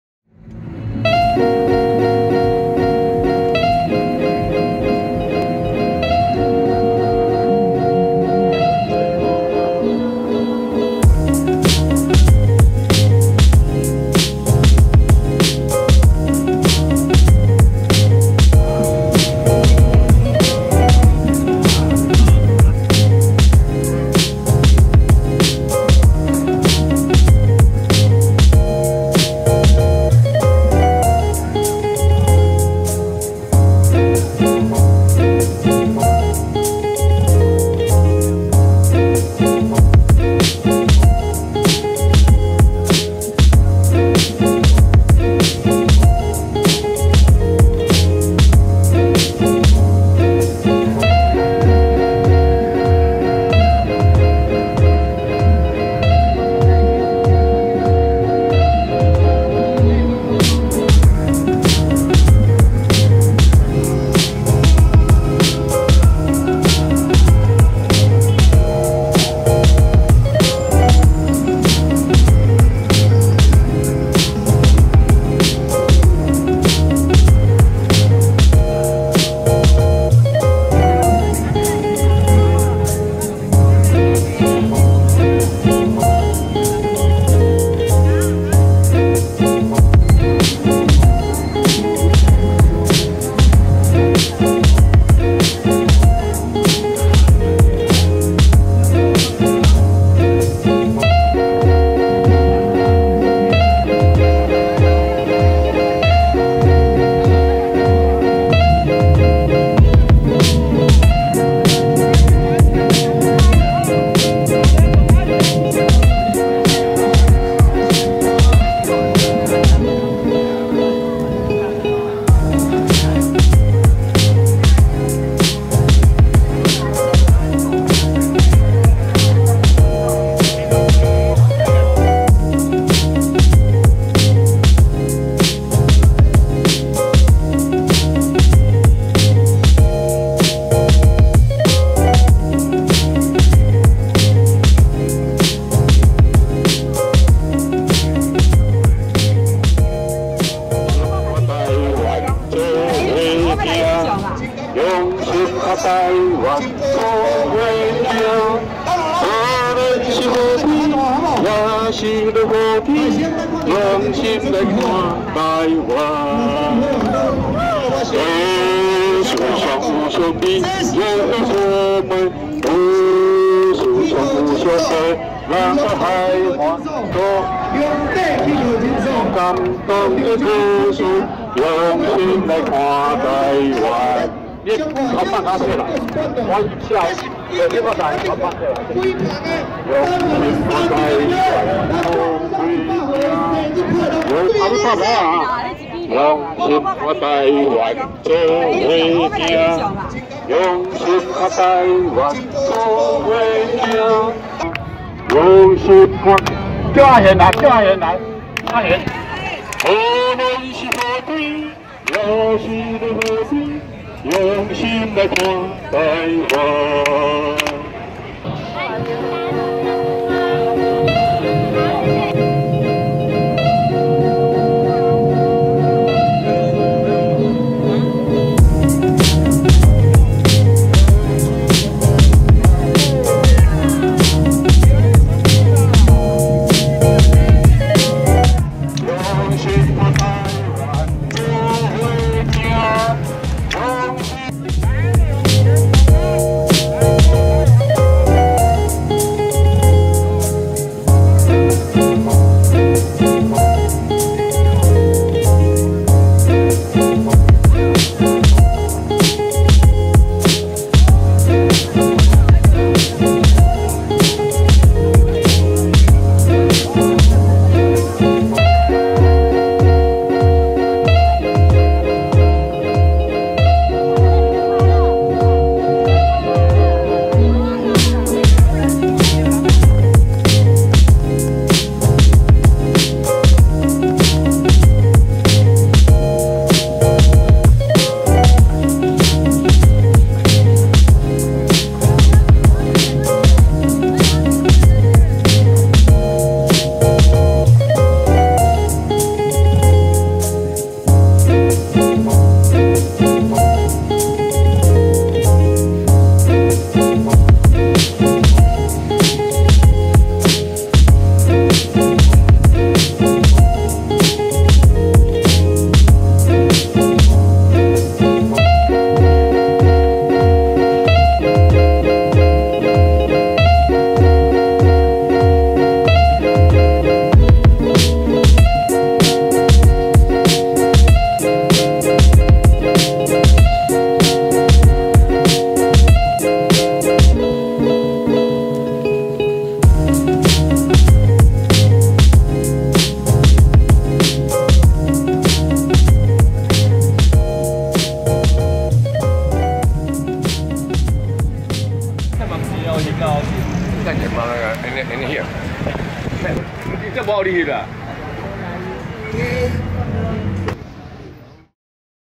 Su Beng's Birthday
Su Beng is a legendary, Taiwanese activist who spent more than 4 decades in exile in Japan. He dedicated his life to the cause and while being unable to come back to his native Taiwan from Japan he still wrote a book about Taiwan's history. This legendary activist had his 100th birthday celebrated in front of the presidential palace in Taipei,